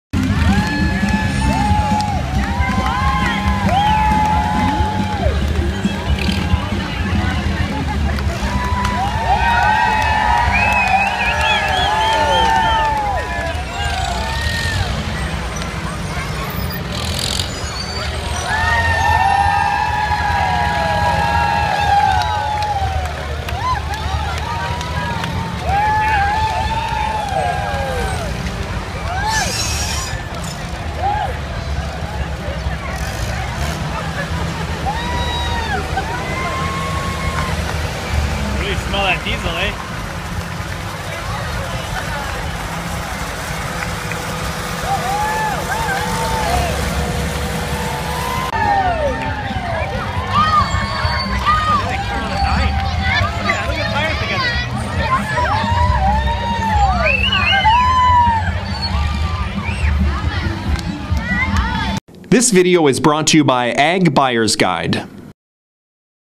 Antique Farmall Tractors Square Dance In A Parade.
Tractor drivers put on a show for the people watching the parade.